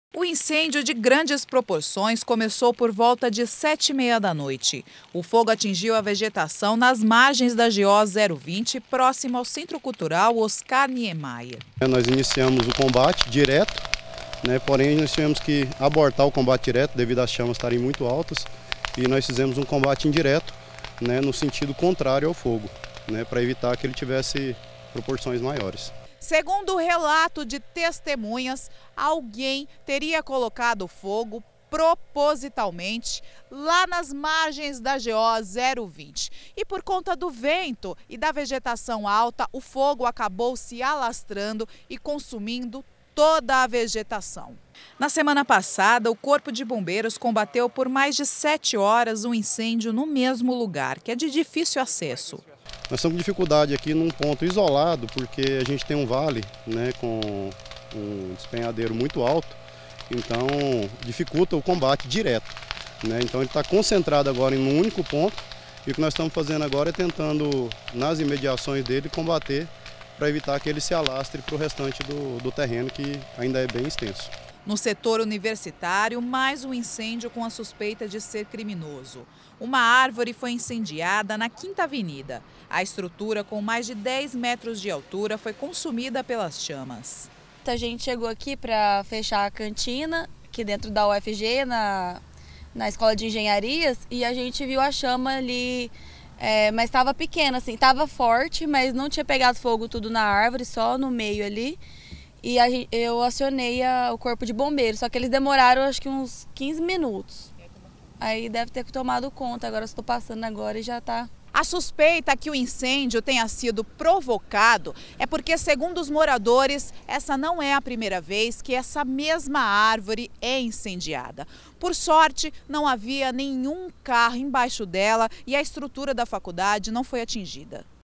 GA - Incêndios em Goiânia - 23-08-2018
Noite de quarta-feira marcada por incêndios em Goiânia. No Setor Universitário uma árvore pegou fogo. Às margens da GO-020, novamente um grande foco alastrou pela vegetação.